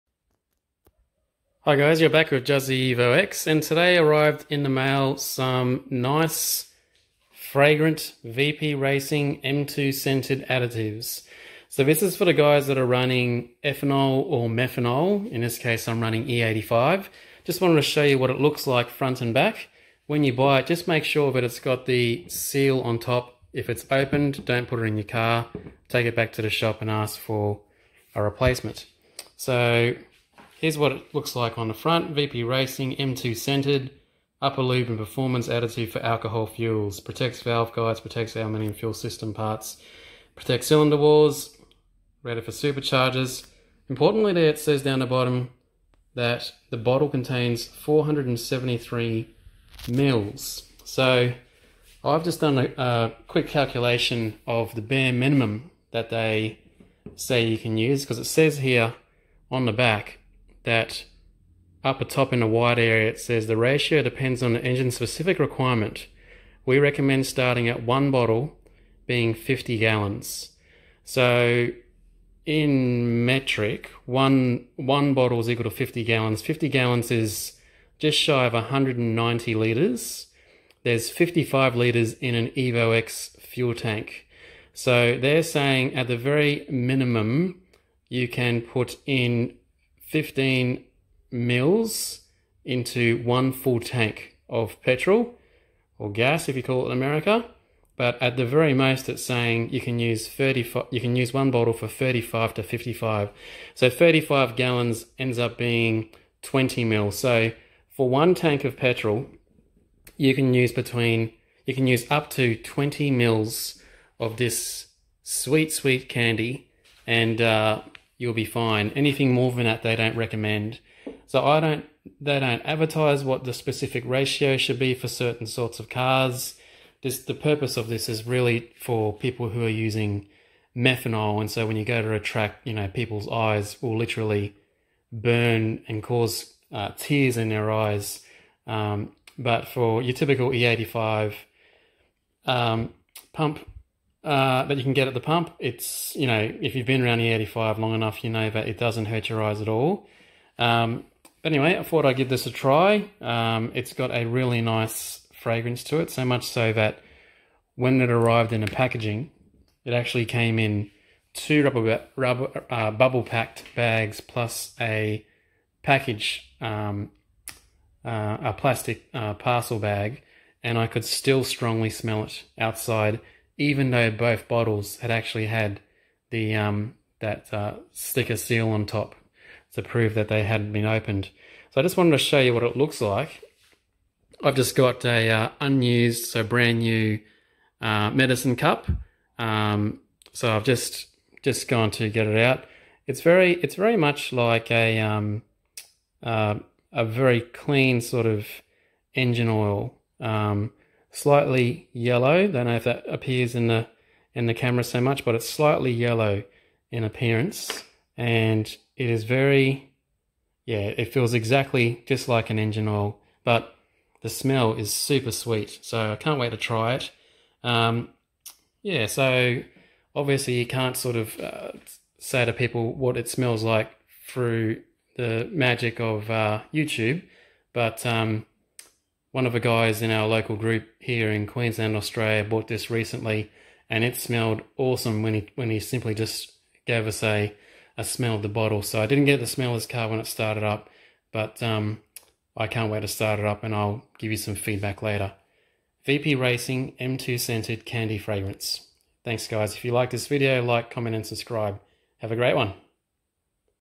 Vp Racing M2 Scented performance additive
Evo X E85 with that sweet candy pop.